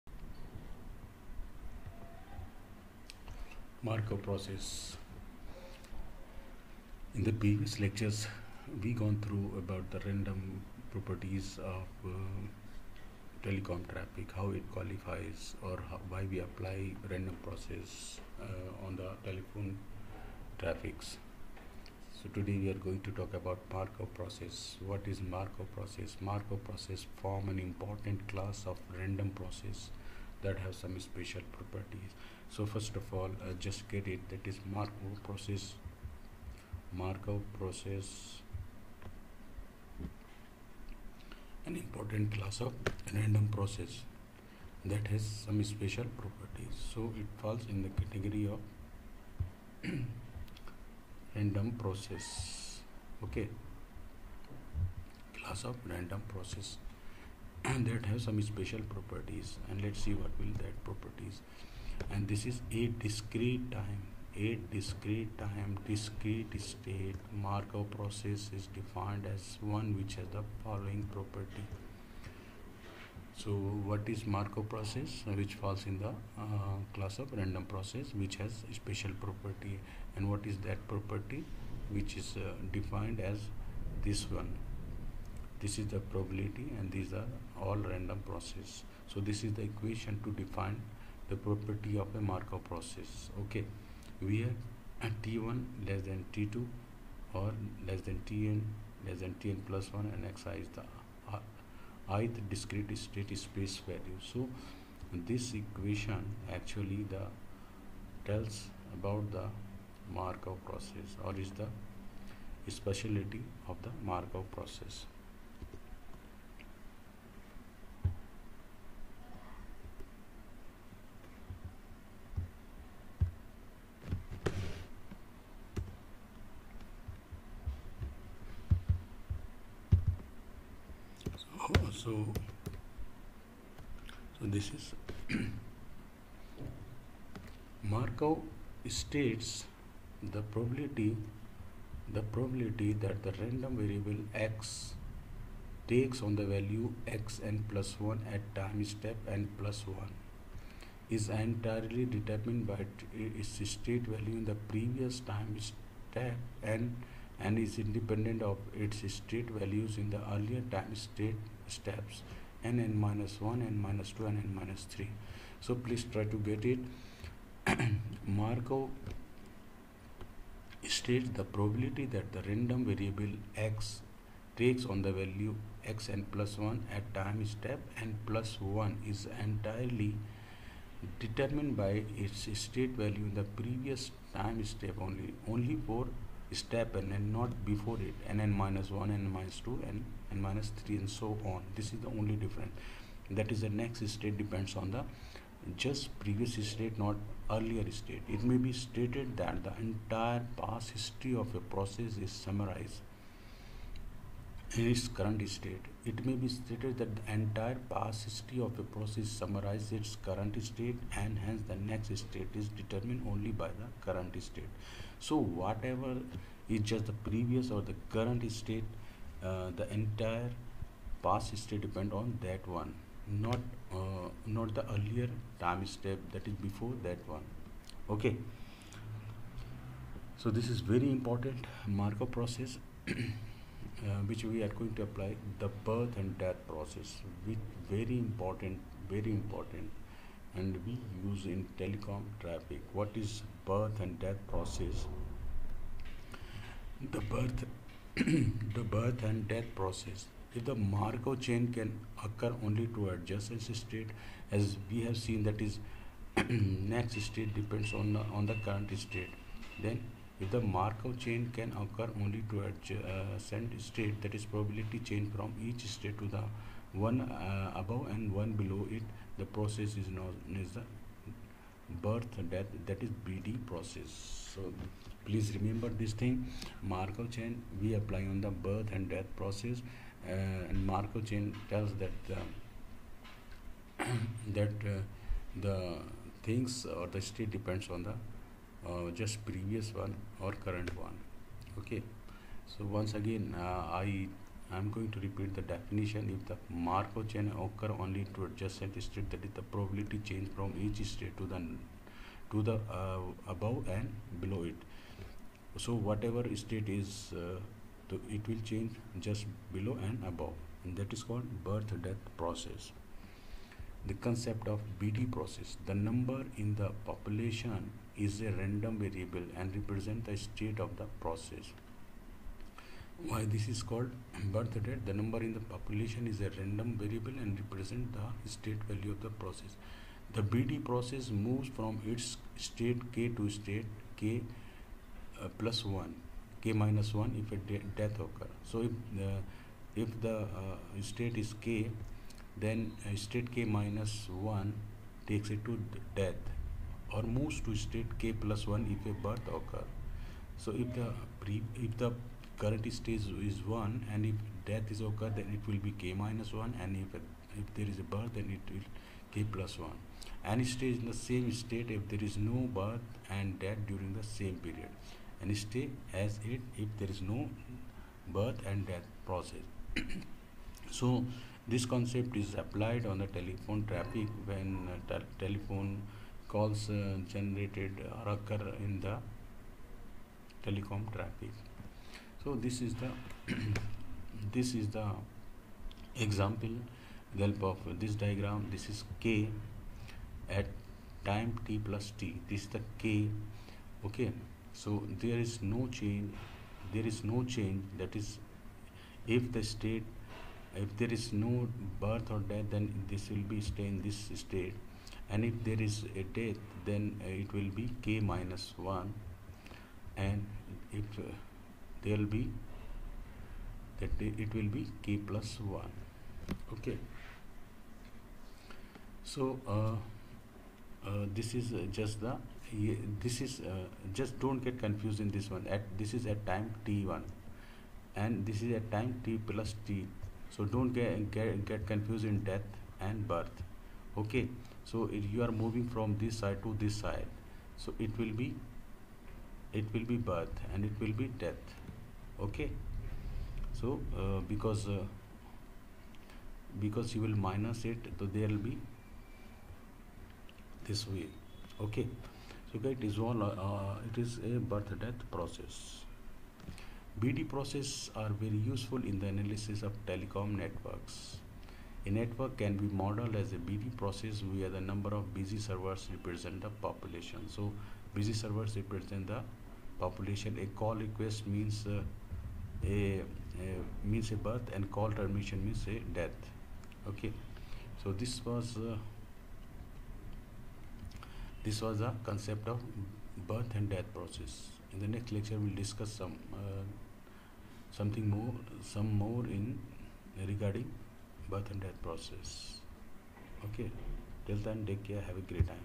Traffic Engineering in telecom networks
These videos explain  what is markov process and why this is so important in telecom  traffic designing engineering